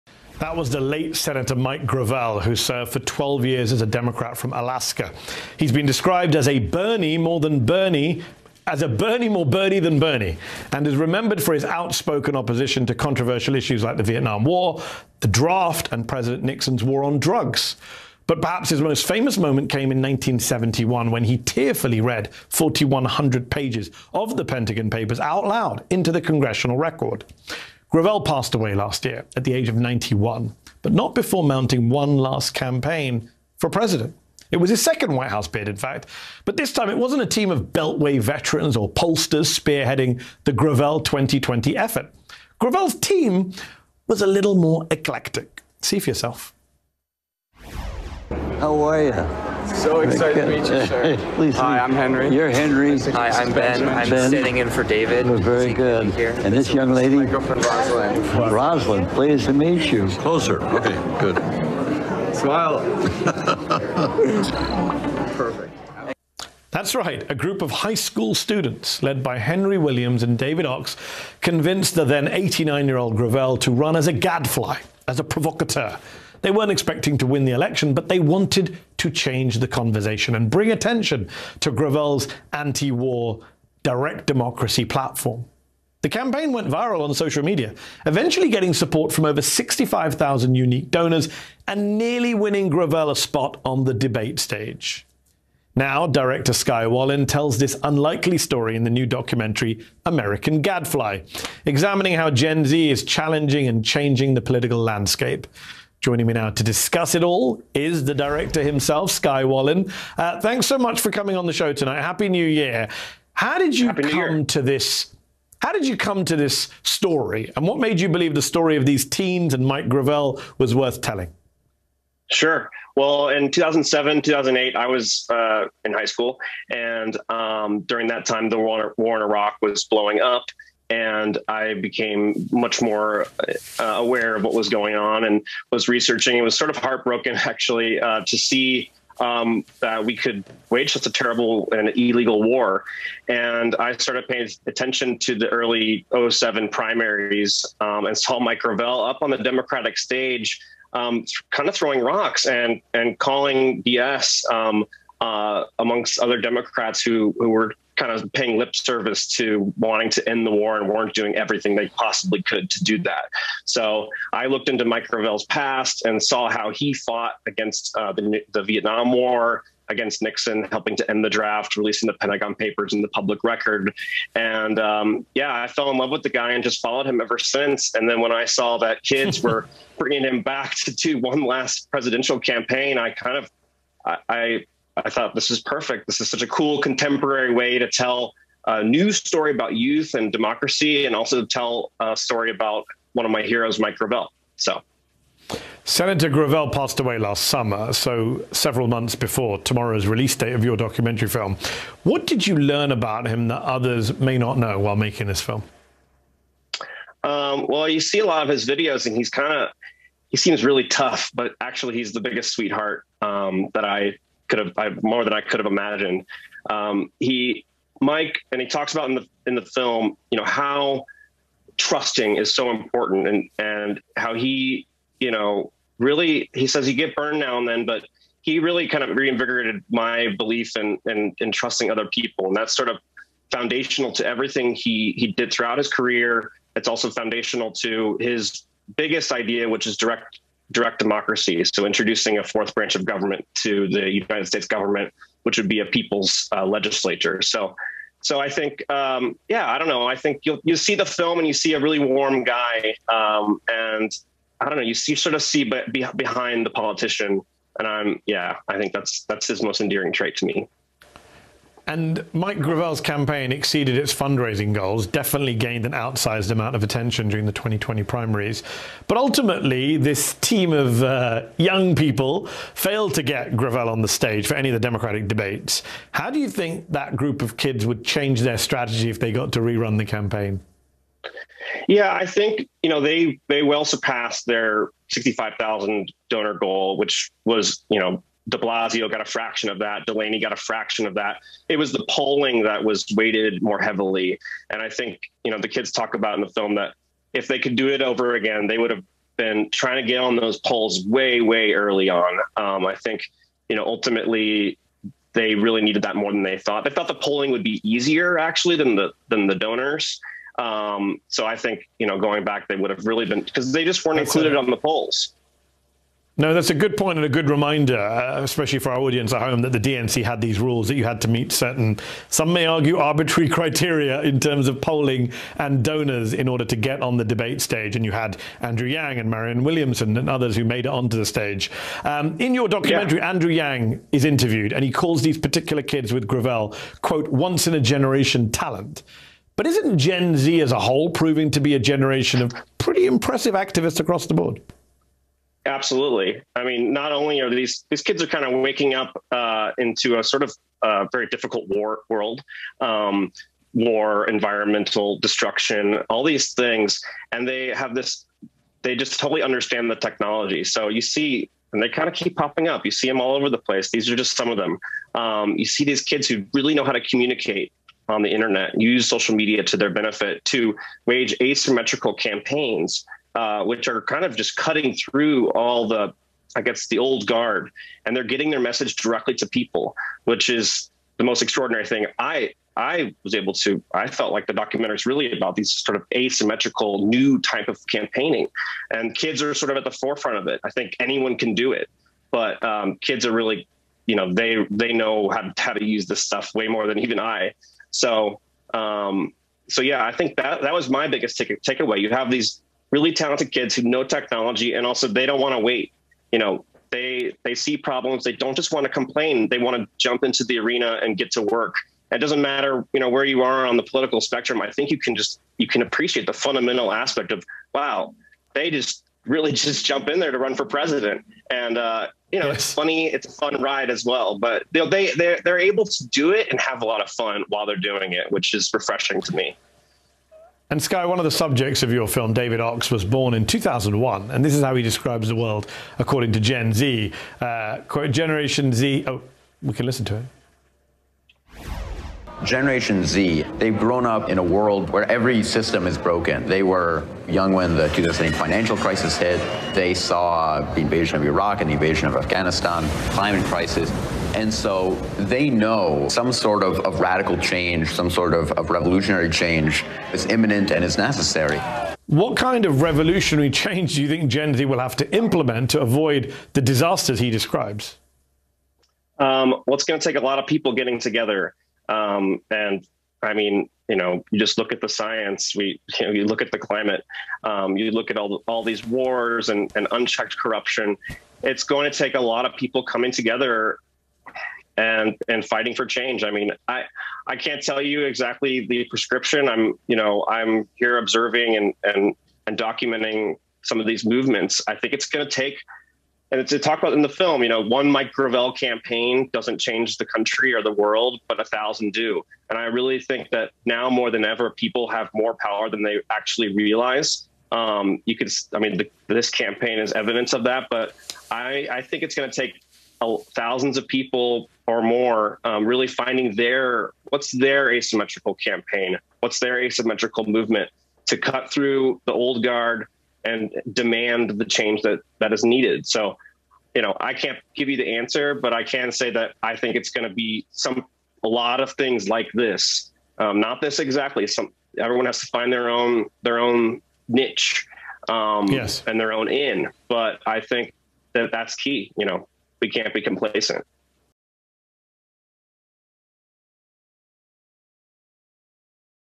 AMERICAN GADFLY Director Speaks with NBC News, Mehdi Hasan
Skye Wallin, Director of AMERICAN GADFLY, speaks with Mehdi Hasan about the late Senator Mike Gravel, his legacy, and his final presidential campaign as run by high school teenagers.